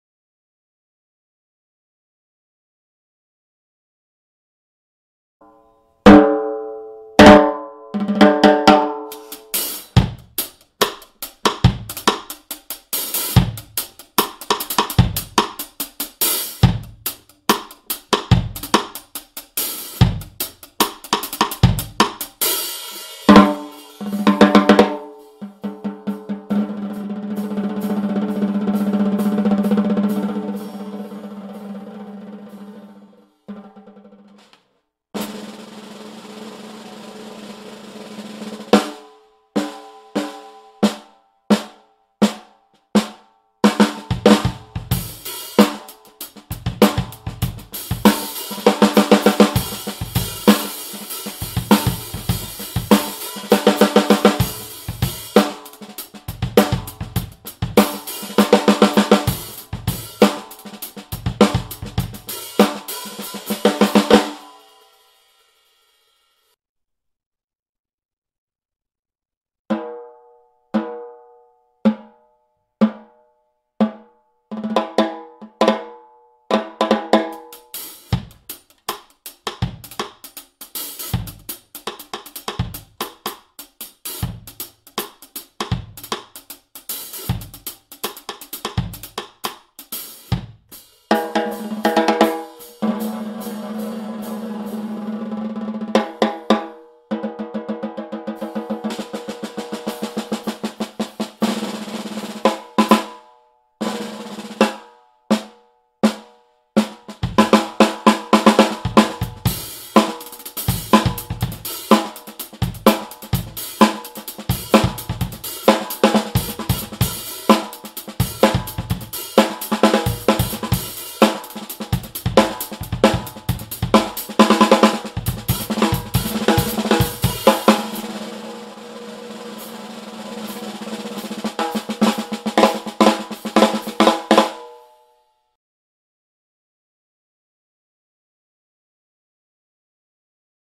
EHC-DRUM 6X14 Purple Heart Stave 1/2" Thick
Here is a Purple Heart Stave drum with Single point lugs, Dunnett strainer, Triple Flange hoops, Pure Sound wires...